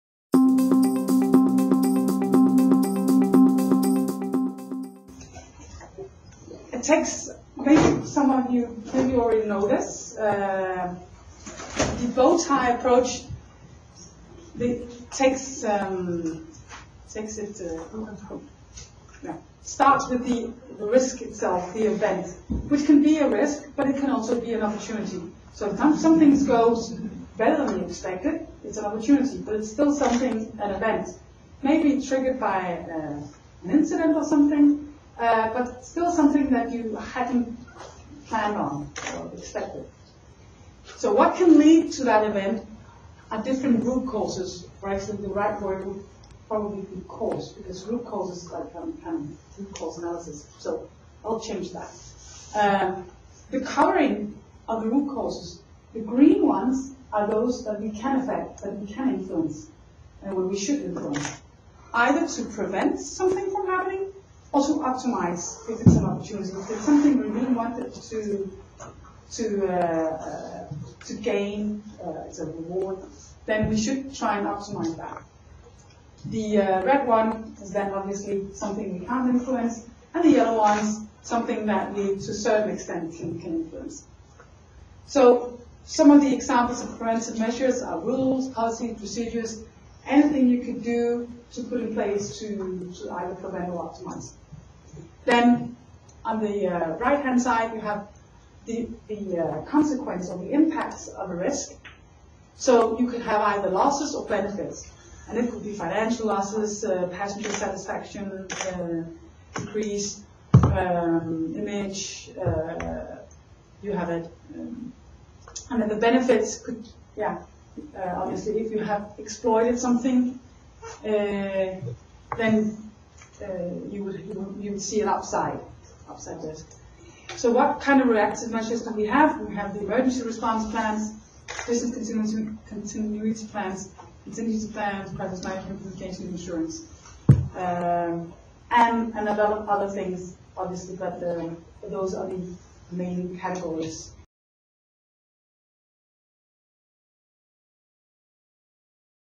CPH’s risk approach using the bow tie model
Copenhagen Airport’s Group Risk Manager, Kristine Raffel, lectures at the University of St. Thomas Opus college of Business on CPH’s risk approach using the bow tie model in risk management.